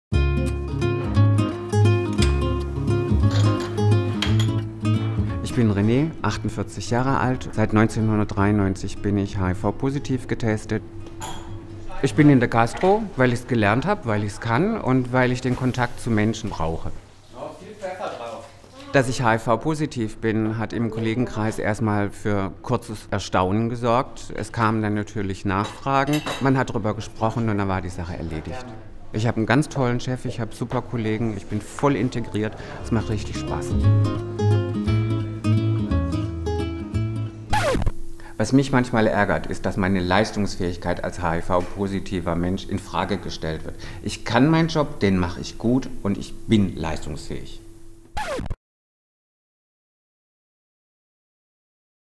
Arbeit und HIV - Selbstverständlich positiv?!
Mit dem Coming Out kennen sich die meisten Schwulen aus. Aber ein HIV-Coming-Out im Job? Das ist nochmal ein anderes Ding. Die wenigsten HIV-Positiven sind an ihrem Arbeitsplatz geoutet. Wir empfehlen: Hört euch dazu Renés Statement "Ich bin leistungsfähig!" an.